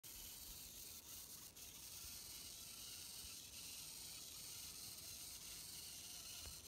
ナローガレージ製Nパワー加工
ナローガレージ製Nパワーにサイドロッドを付けて加工したものの走行動画です。